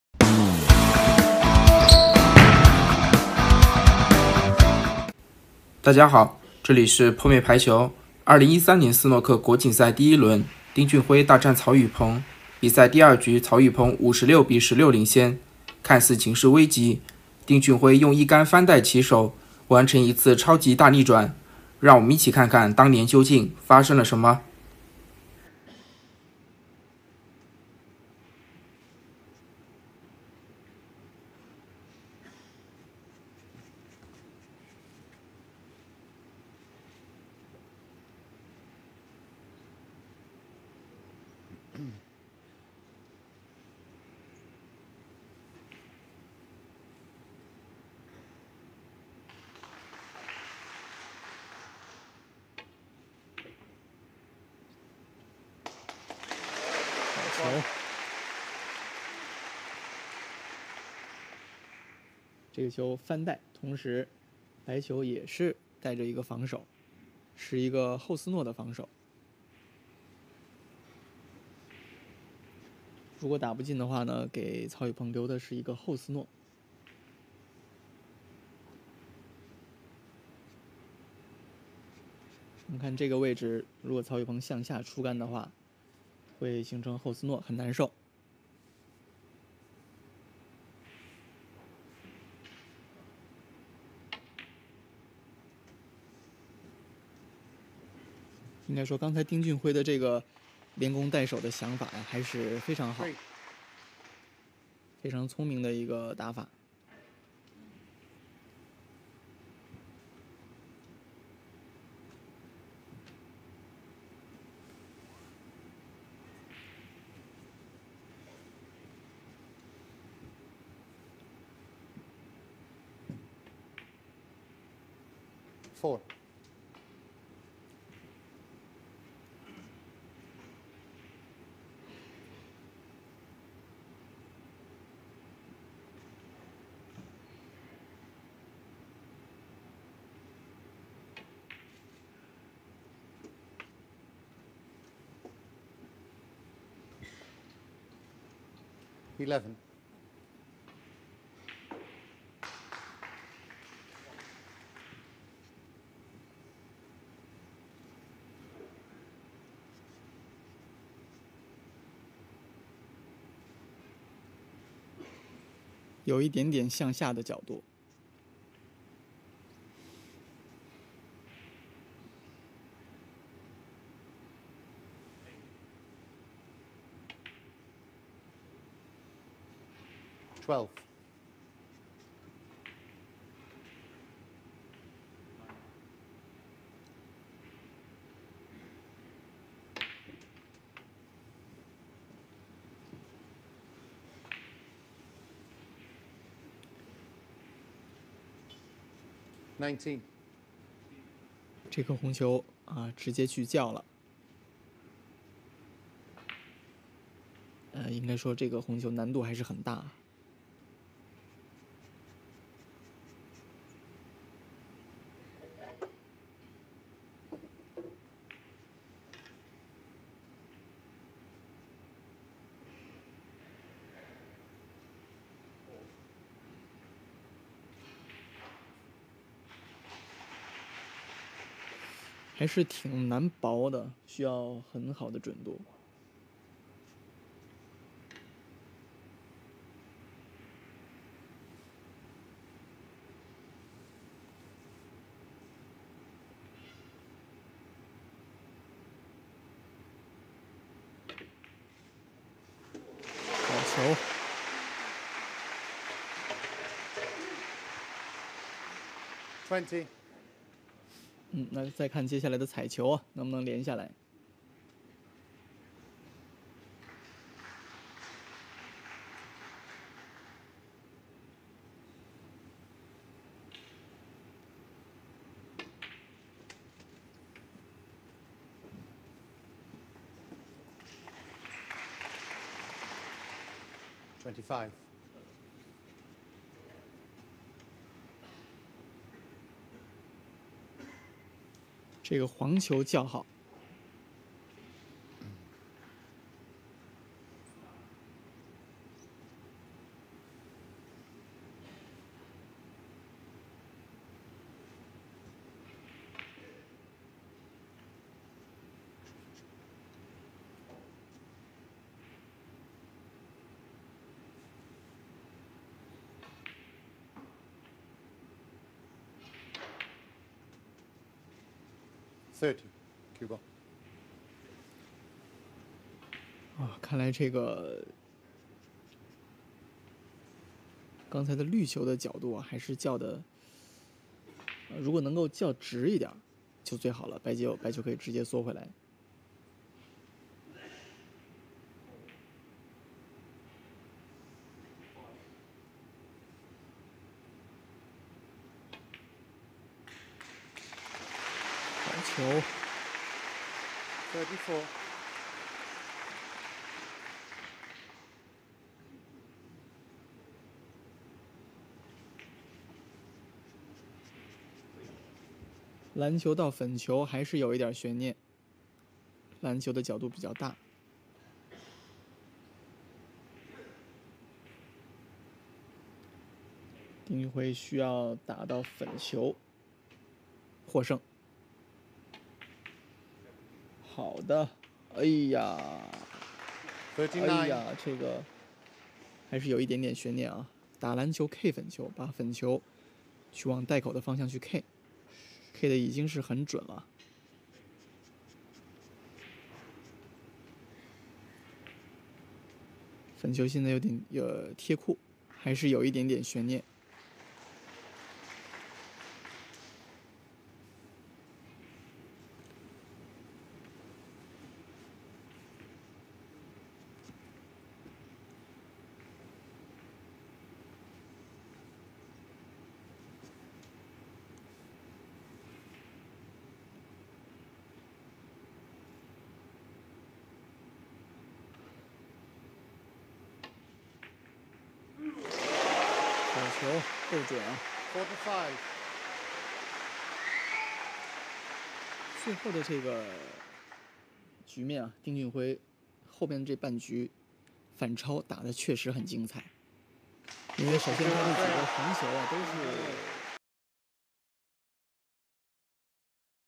丁俊晖一杆翻袋起手，引发16-56超级逆转，最后粉球考验球迷心脏
丁俊晖一杆翻袋起手，引发16-56超级逆转，最后粉球考验球迷心